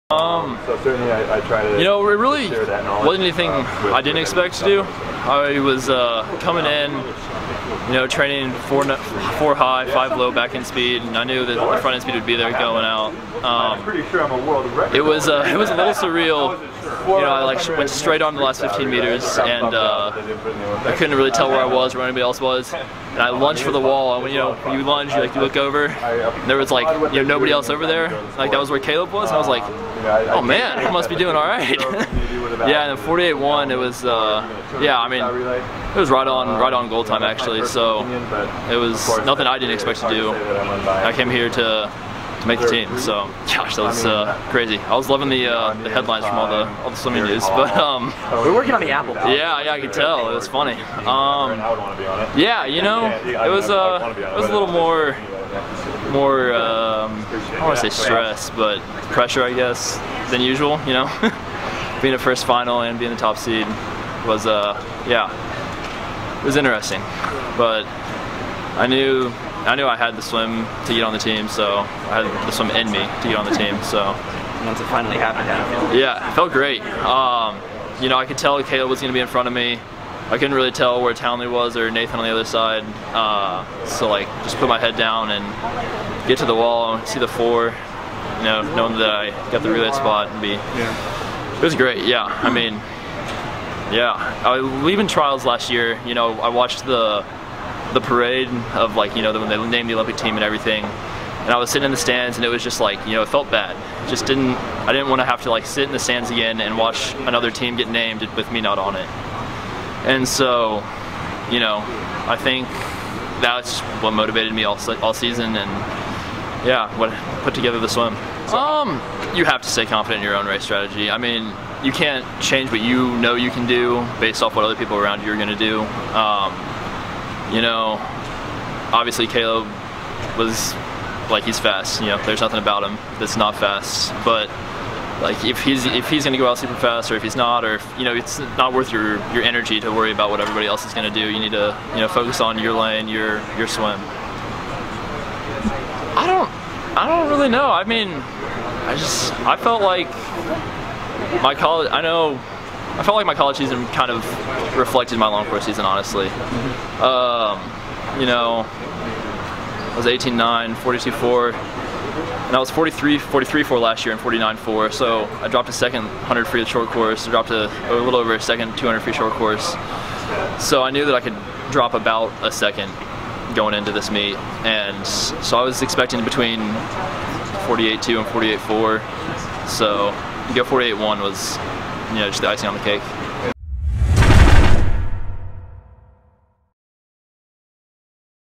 Zach Apple Talks 48.1 100 Freestyle And Making The World Team
Zach Apple, despite his recent success at the NCAA Swimming and Diving Championships, made headlines across the swimming community asking "who is this guy?"...we know who he is. He's one of your 400 freestyle relay members for Team USA at Worlds.